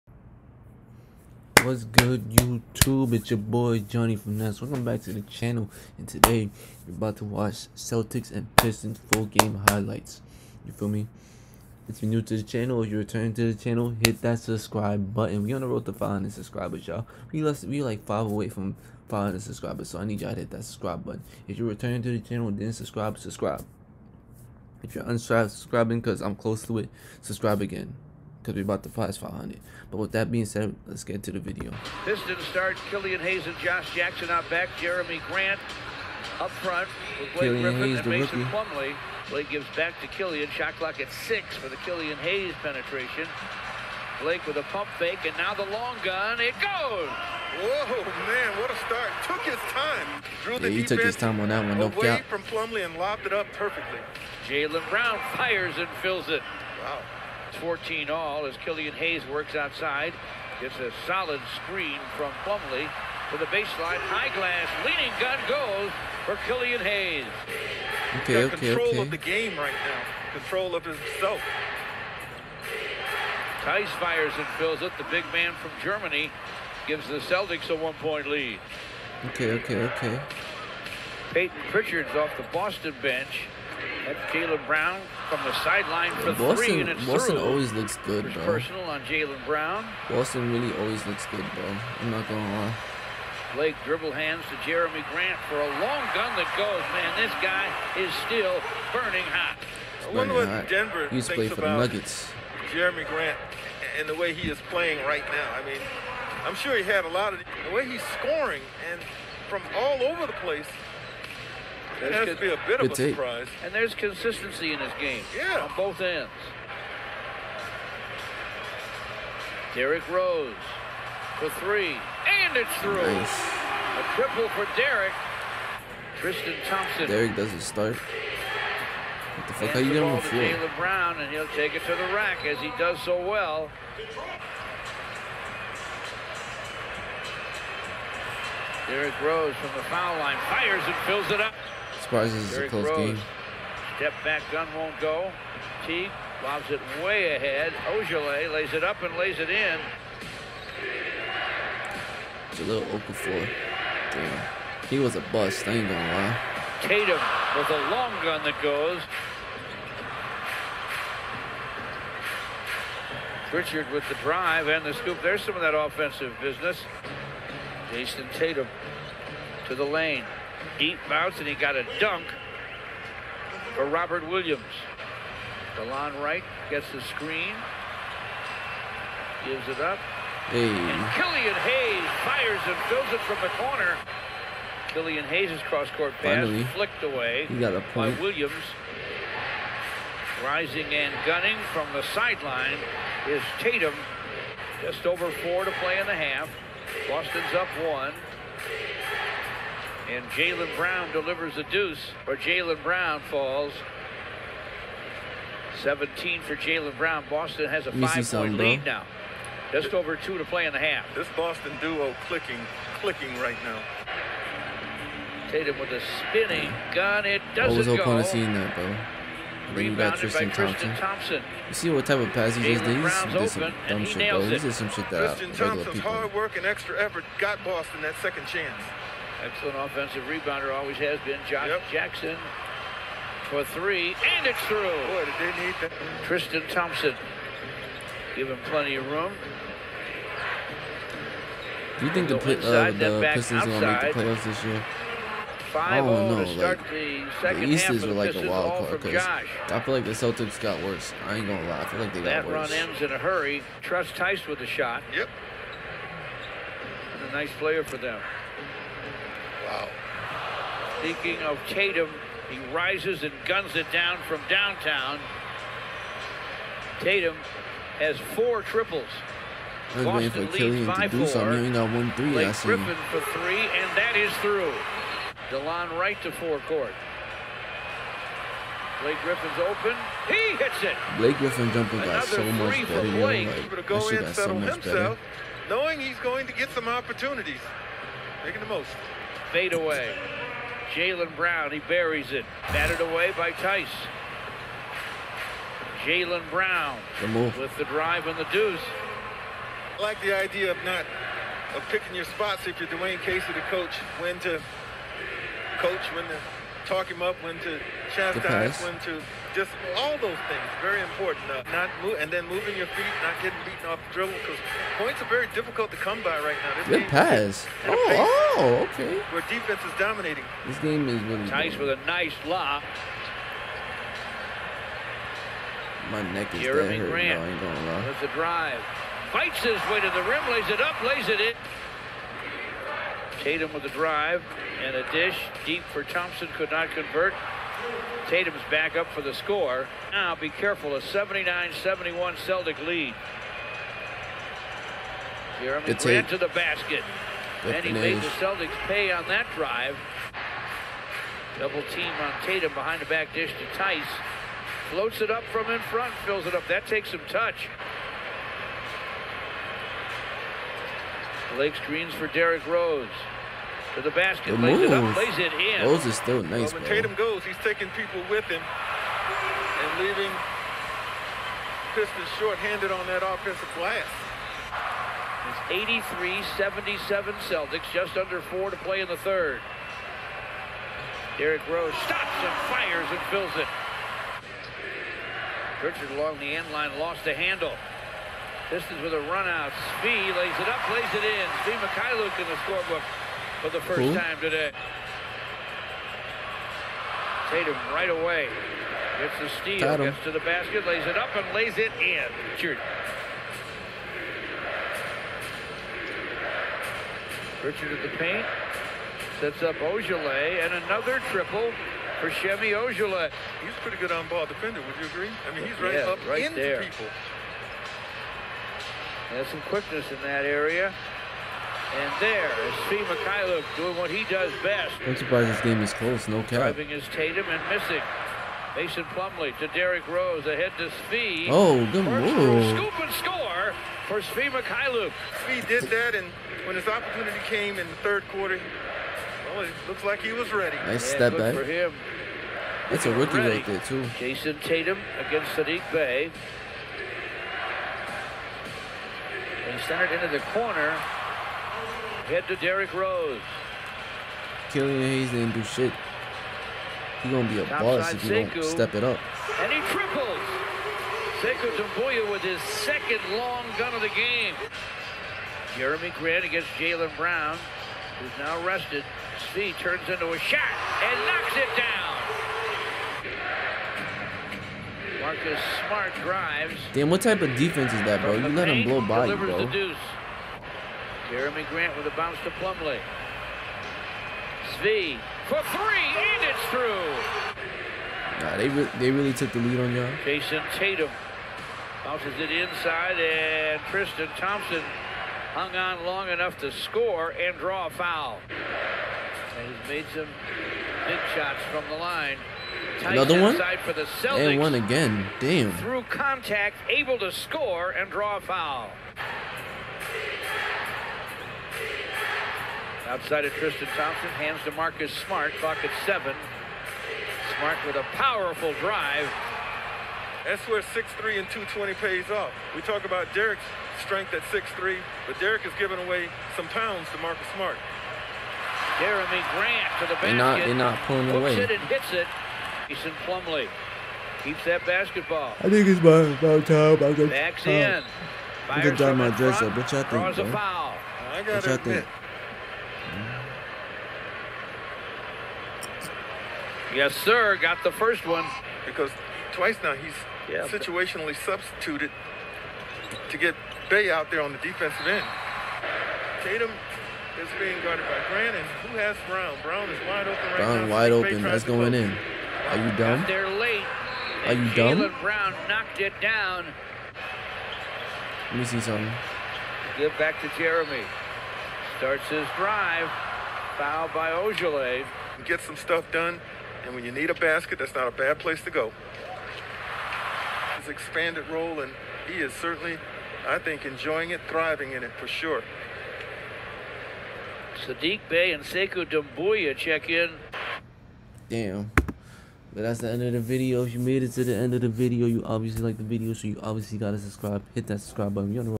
GAME WINNER CELTICS at PISTONS | FULL GAME HIGHLIGHTS | January 3, 2021 Johnny Finesse Reaction
My microphone:
Blue Snowball USB Microphone (Brushed Aluminum)